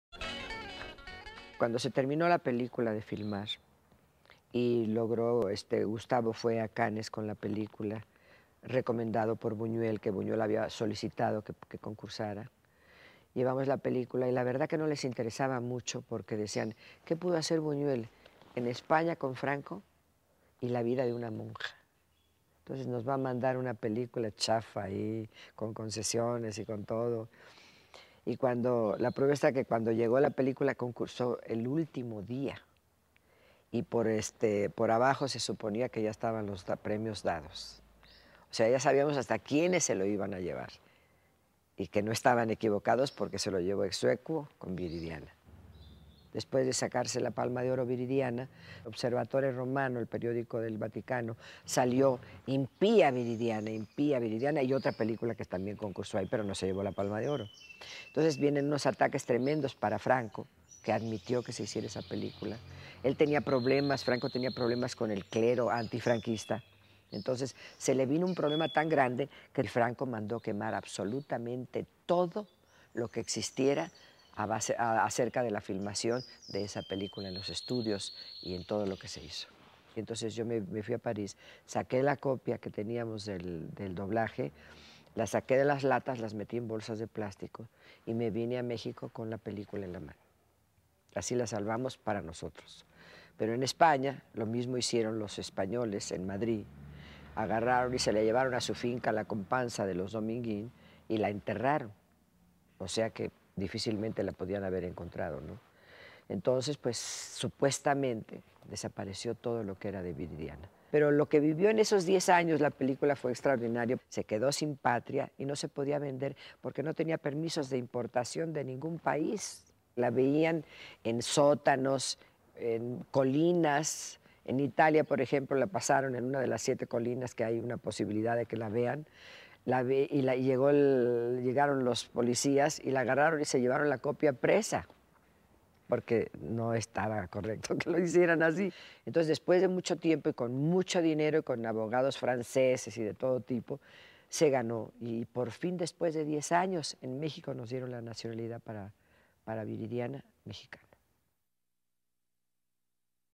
Silvia Pinal on Viridiana
Silvia Pinal discusses the Viridiana premier and banning of the film by Francisco Franco